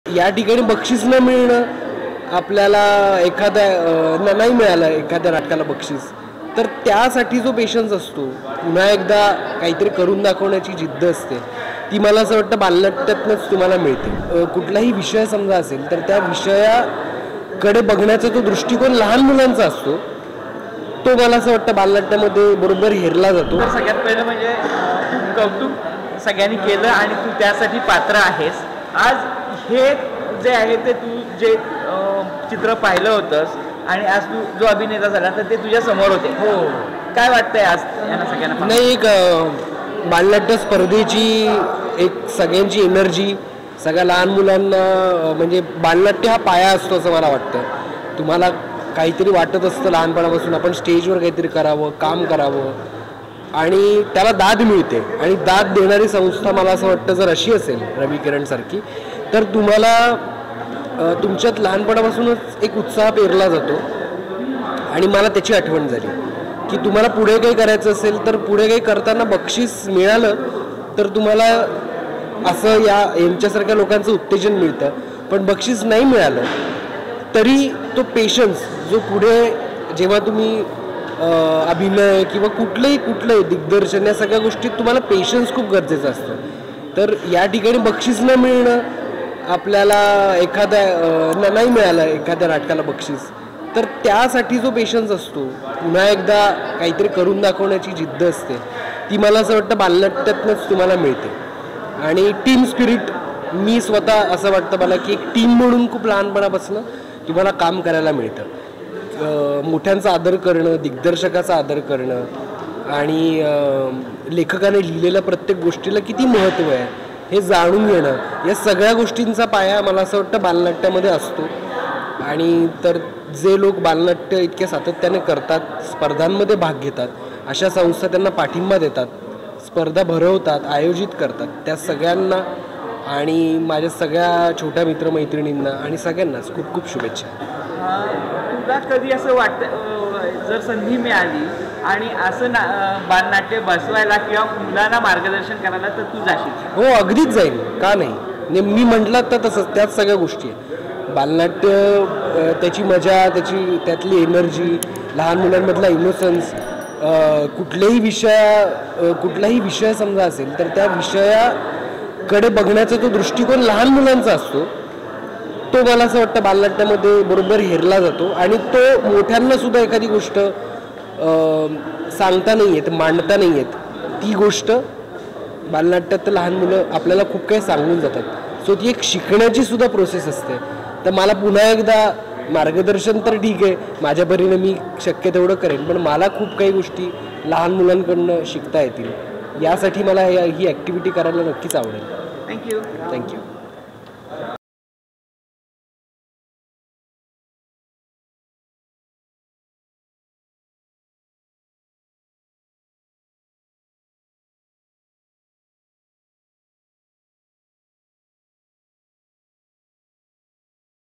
Abhishek Deshmukh : मुलांमधील कलागुणांना वाव मिळण्यासाठी, बालनाट्य स्पर्धा उत्तम पर्याय | LetsUpp
रविकिरण बालनाट्य स्पर्धेमध्ये अभिनेता अभिषेक देशमुखने केले बालकलाकारांचे कौतुक